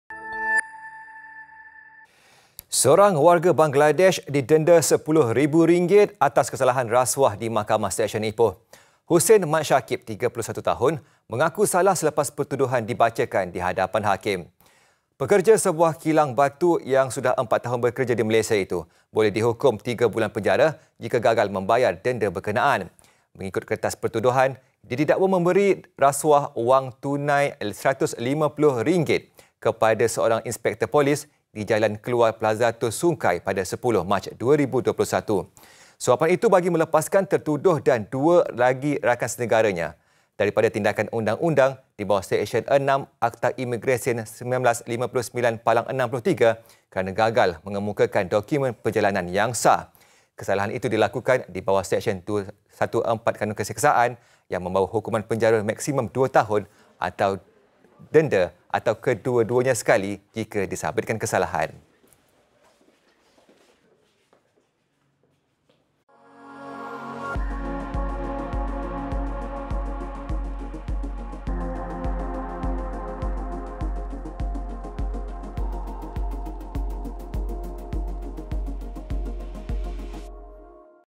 MAHKAMAH | Warga Bangladesh Didenda RM10,000 Cuba Sogok Polis
Seorang warga Bangladesh didenda RM10,000 atas kesalahan rasuah di Mahkamah Seksyen Ipoh.

Hossain Md Shakib, 31 tahun, mengaku salah selepas pertuduhan dibacakan di hadapan hakim.